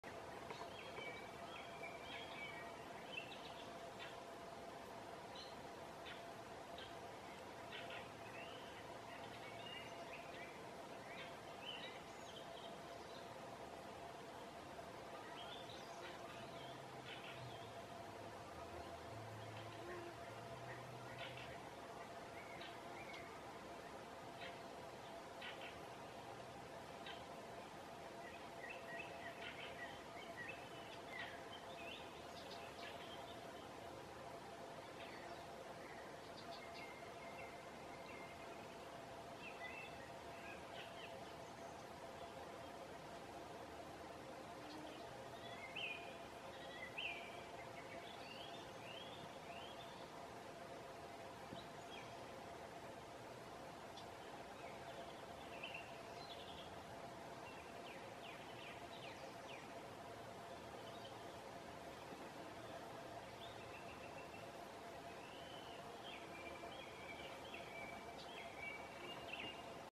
It's almost calm, but aspen leaves keep rustling
Haavanlehdet havisevat, vaikka on melkein tyyntä.